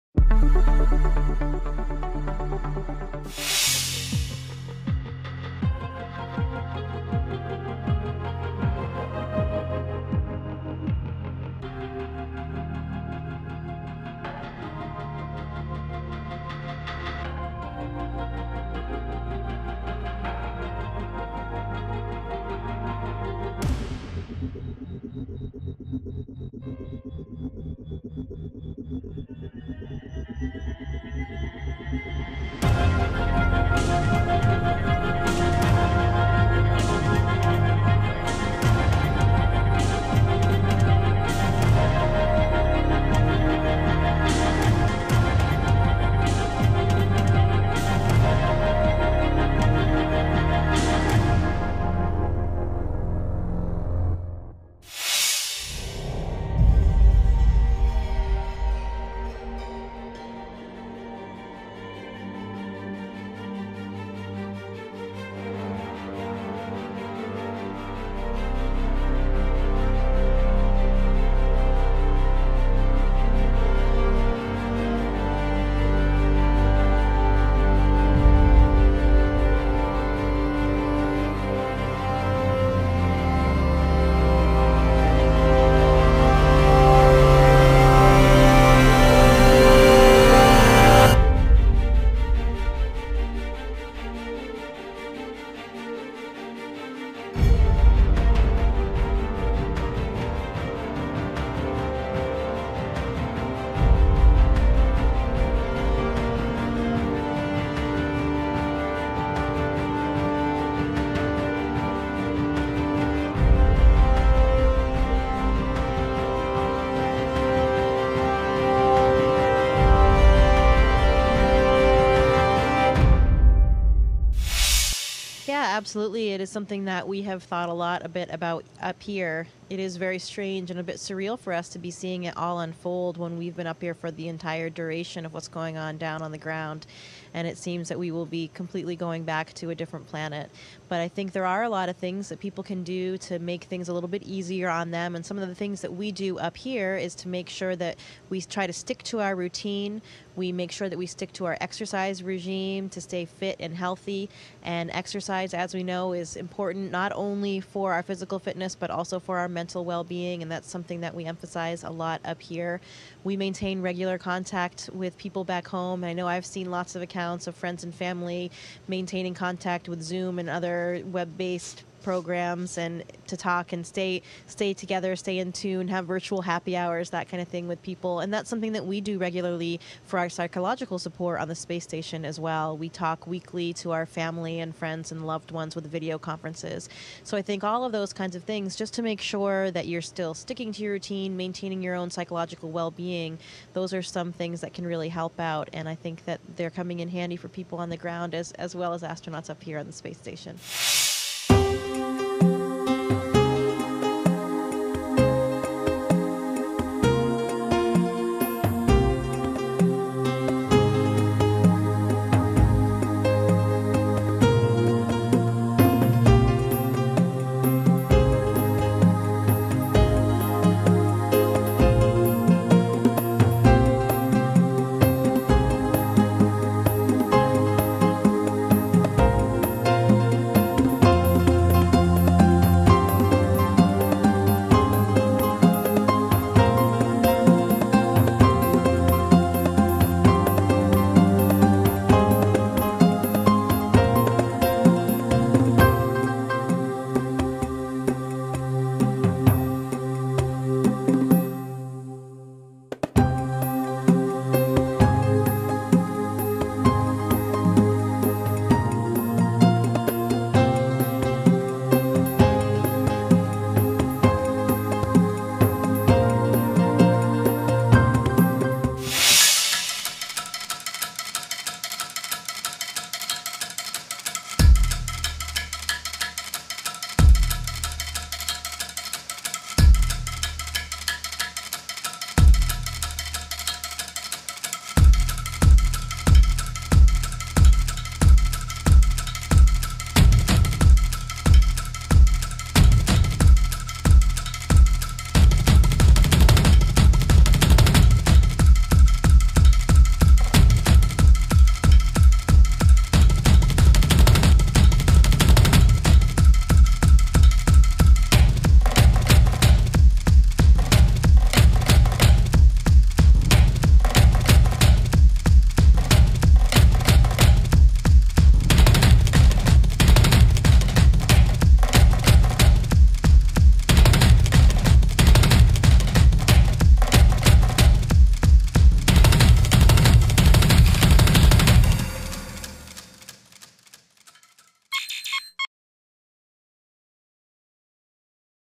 Video from Space - Weekly highlights: Week of March 29, 2020
China continued work on their new crew spacecraft, the Milky Way’s warped disk has been studied using ESA’s Gaia mission, NASA astronaut Jessica Meir gave some tips on staying healthy at home, air pollution over the United States declined significantly in March and SpaceX’s Starship SN3 prototype failed during a test.

Related Stories:

China's new crew spacecraft looks like it could dock with the International Space Station

A galactic crash could be behind Milky Way's warped appearance

'Going back to a different planet' - Astronaut Jessica Meir

Shutdowns from coronavirus create blue skies in California, could inform future pollution control

SpaceX's Starship SN3 prototype collapses in pressure tank test